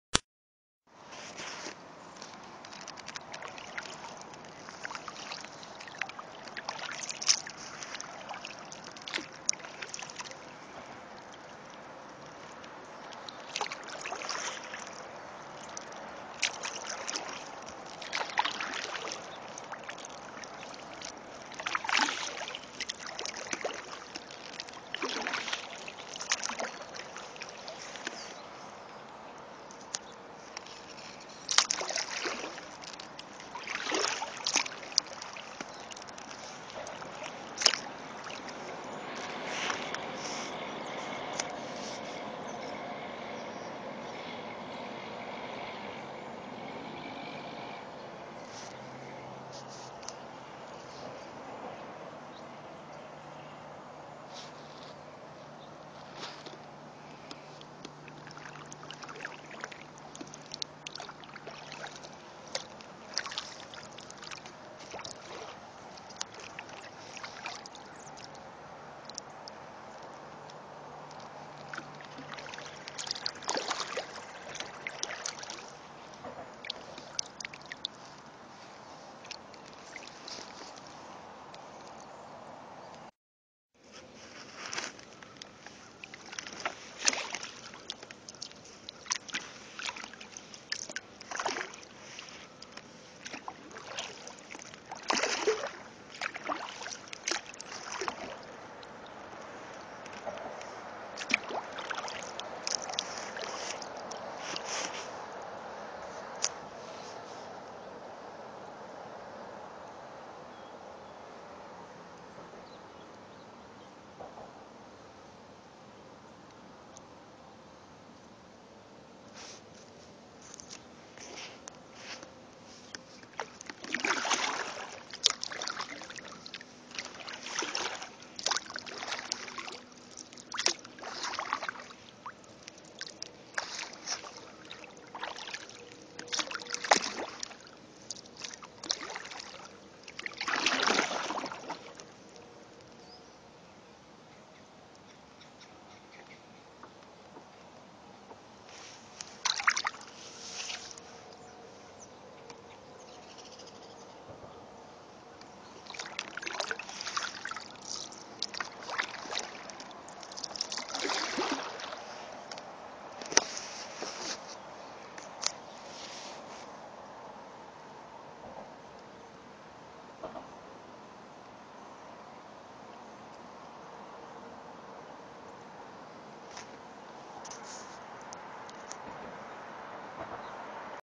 Foggy Kayaking on the Wabash.
A little foggy morning on the Wabash River.